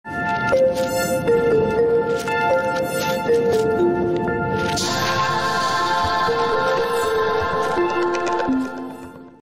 ERT World - Summer Ident #1 (2023-2024)
Ένα από τα καλοκαιρινά ident της ERT World για το 2023 και το 2024.